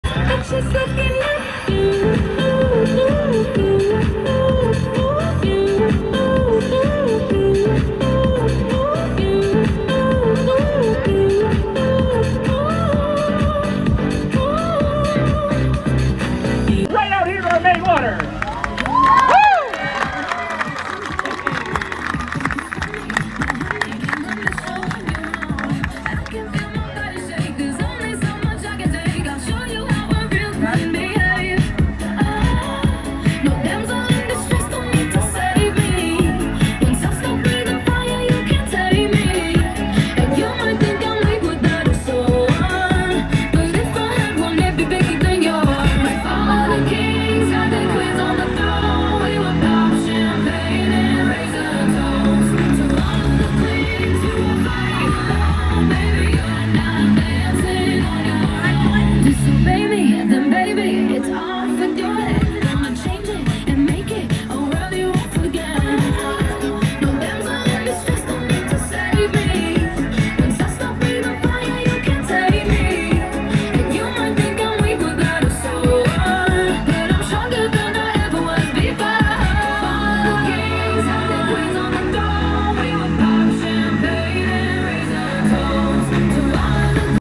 The Aberdeen Aqua Addicts Waterski Ballet
The ballet team hits the water at the Midwest Show Ski tournament.